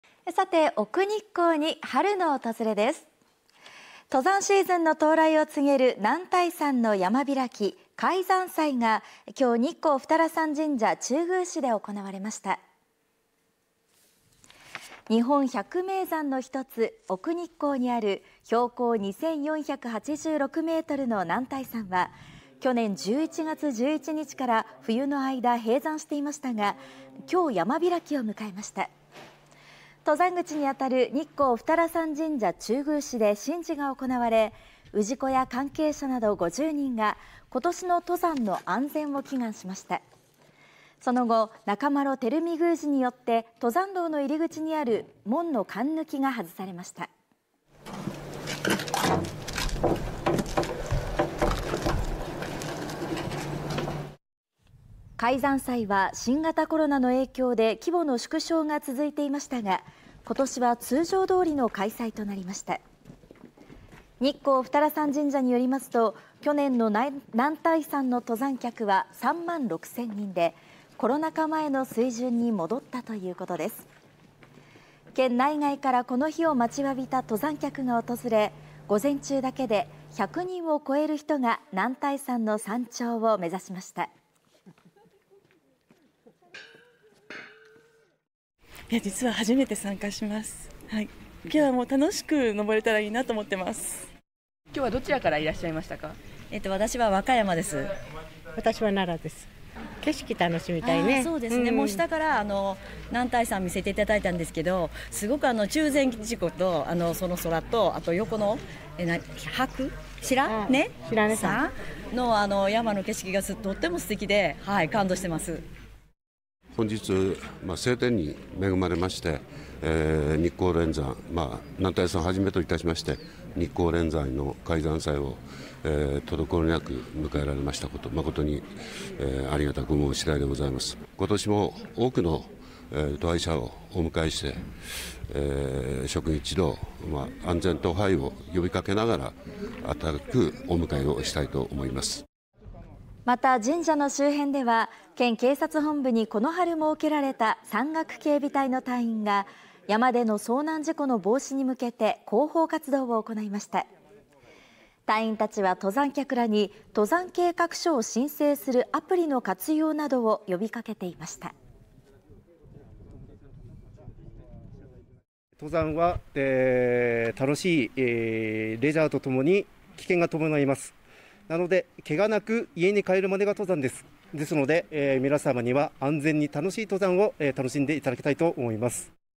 奥日光に登山シーズン到来 男体山の開山祭
奥日光に春の訪れです。登山シーズンの到来を告げる男体山の山開き「開山祭」が４月２５日、日光二荒山神社中宮祠で行われました。

　日本百名山のひとつ、奥日光にある標高２４８６メートルの男体山は２０２３年１１月１１日から冬の間閉山していましたが、４月２５日に山開きを迎えました。

　登山口にあたる日光二荒山神社中宮祠で神事が行われ、氏子や関係者など５０人が今年の登山の安全を祈願しました。その後、中麿　輝美宮司によって登山道の入り口にある門のかんぬきが外されました。

　開山祭は新型コロナの影響で規模の縮小が続いていましたが、今年は通常通りの開催となりました。

　日光二荒山神社によりますと、２０２３年の男体山の登山客は３万６千人でコロナ禍前の水準に戻ったということです。栃木県内外からこの日を待ちわびた登山客が訪れ、午前中だけで１００人を超える人が男体山の山頂を目指しました。

　また、神社の周辺では栃木県警察本部にこの春設けられた山岳警備隊の隊員が山での遭難事故の防止に向けて広報活動を行いました。隊員たちは登山客らに登山計画書を申請するアプリの活用などを呼びかけていました。